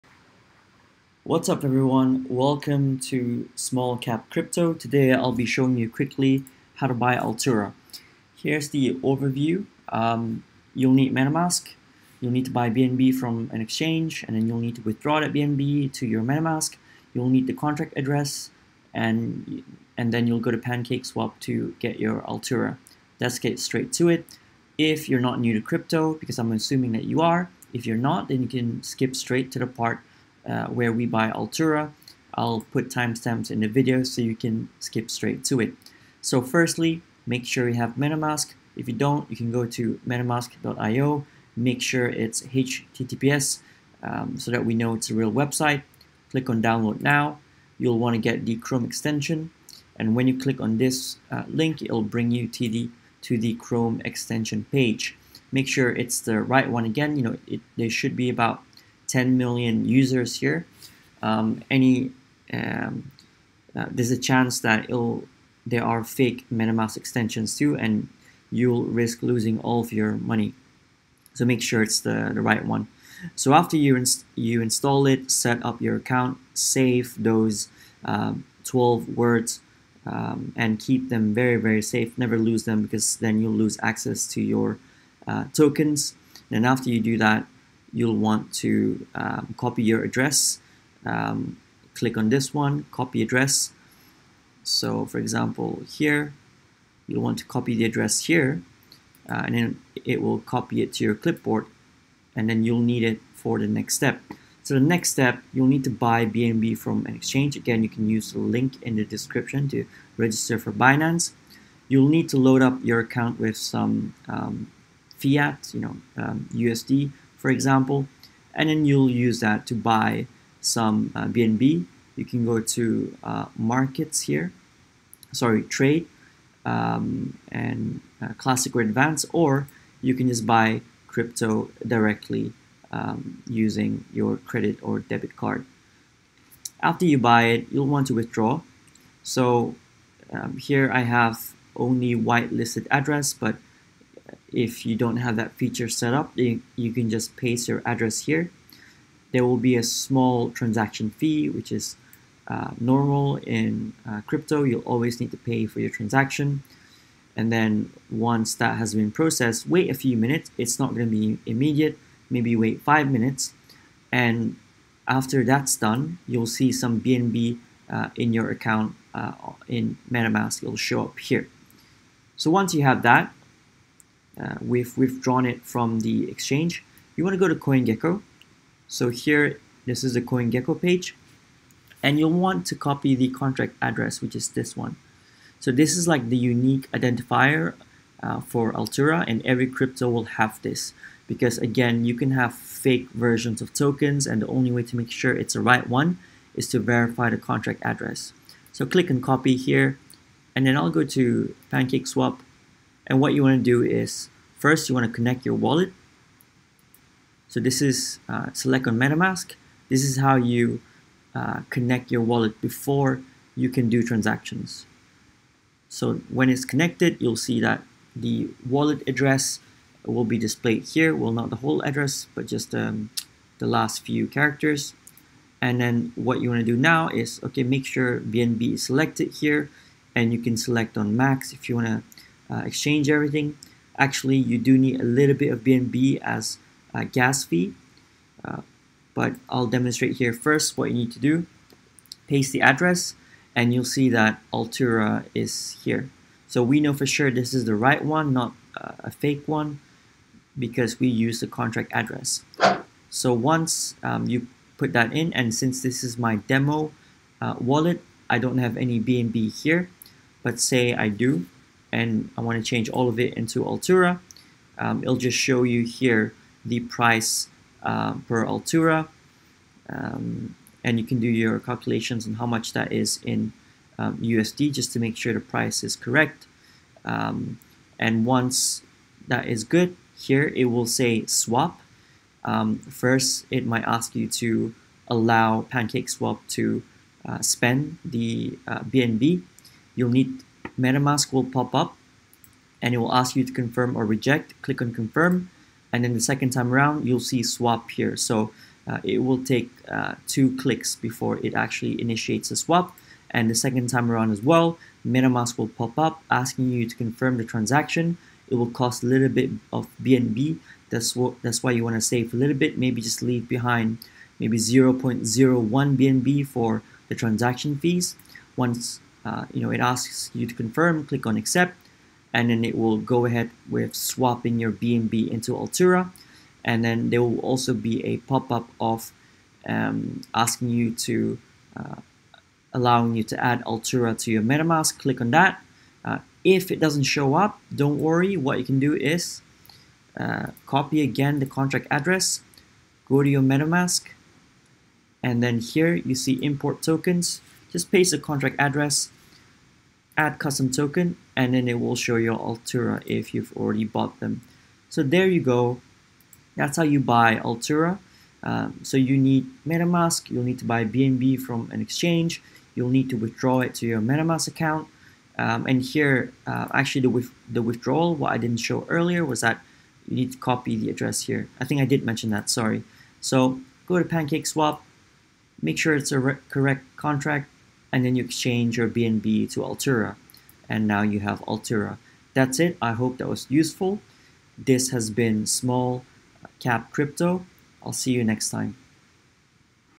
How to buy Altura (ALU) using PancakeSwap, simple guide
00:38 Setup MetaMask
01:41 Buy BNB from an exchange
02:20 Withdraw BNB to MetaMask
02:58 Get the contract address
03:30 PancakeSwap: connect wallet [skip to this step if you already have BNB in your MetaMask]
04:00 PancakeSwap: past contract address
04:20 PancakeSwap: swap!